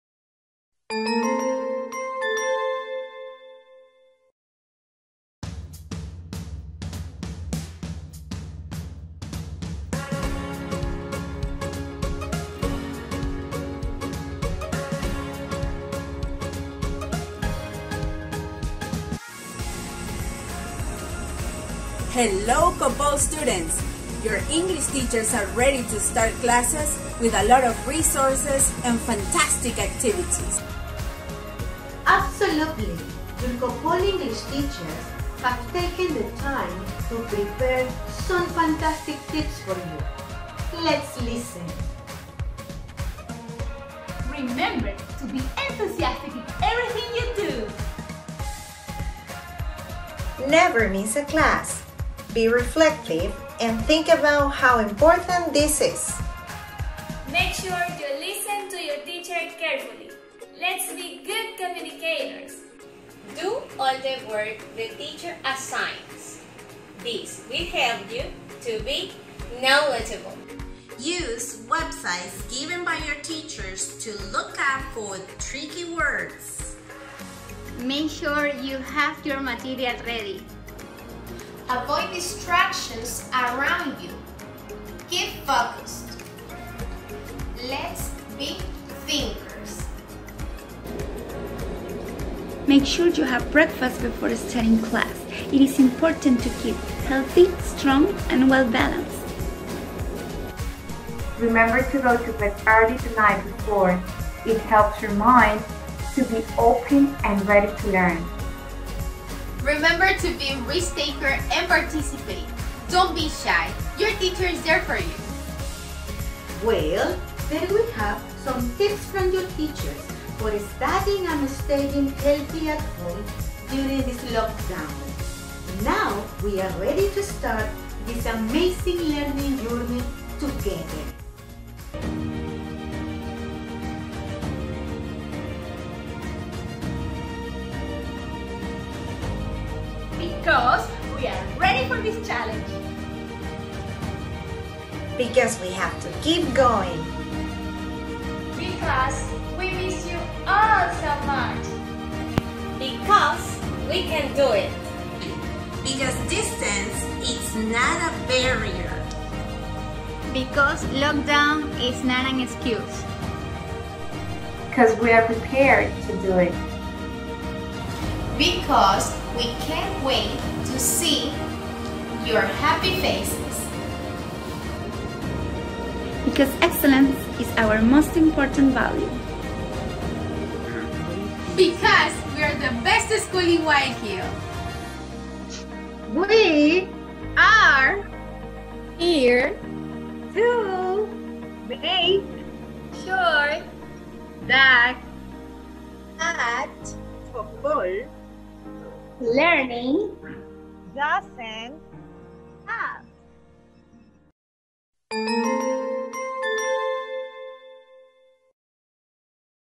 The Primary School English Department
The primary English team have prepared a motivating clip with very useful English learning tips for our students.